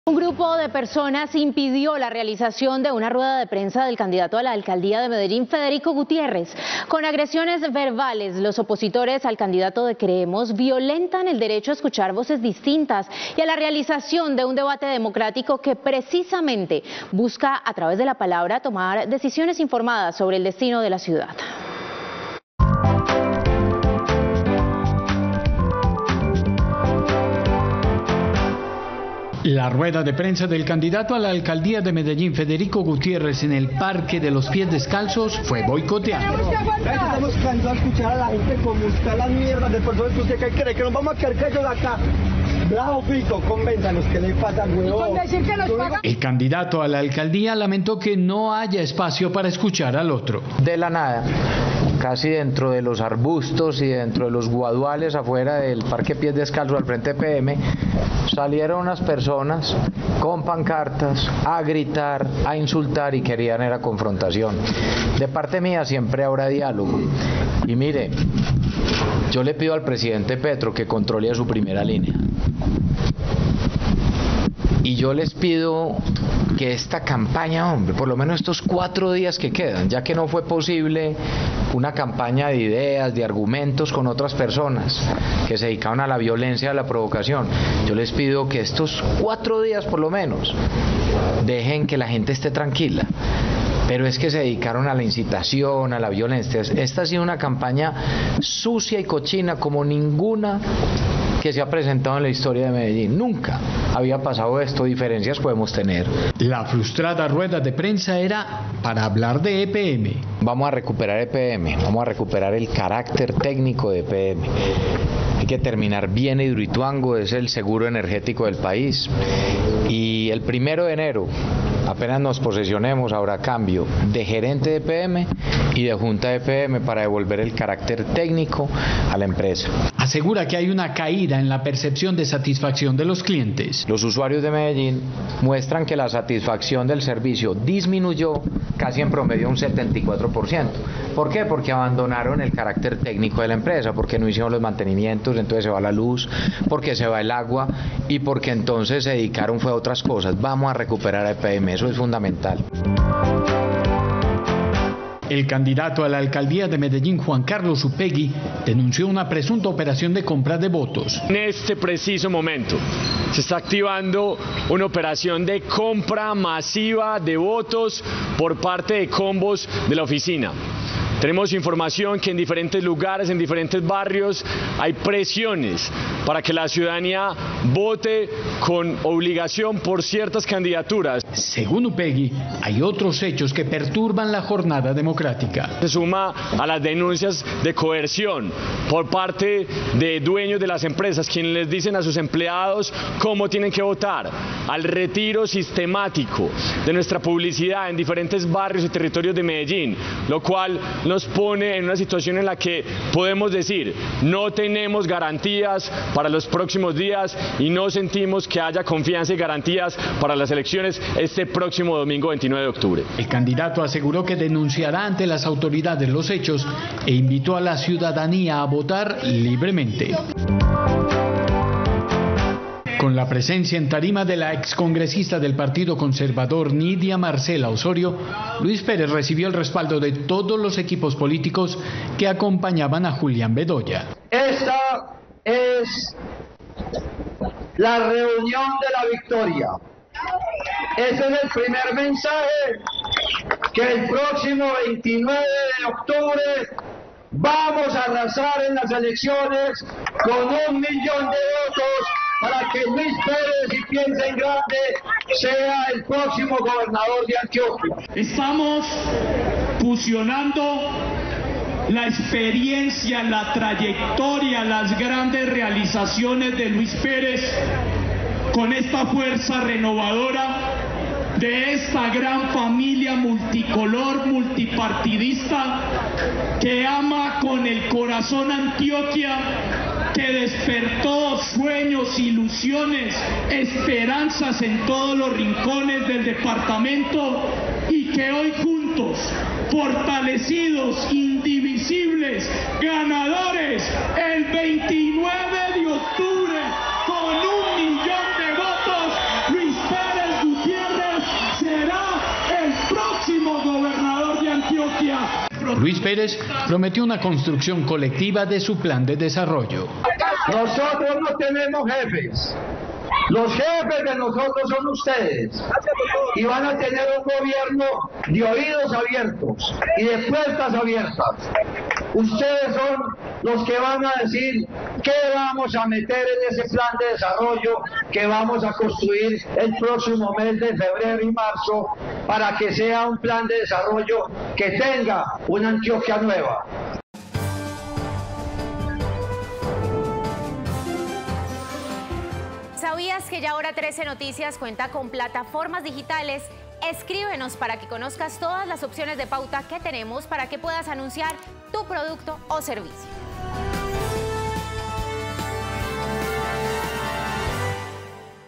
Un grupo de personas impidió la realización de una rueda de prensa de un candidato a la alcaldía
Con agresiones verbales, los opositores al candidato de creemos violentan el derecho a escuchar voces distintas y a la realización de un debate democrático.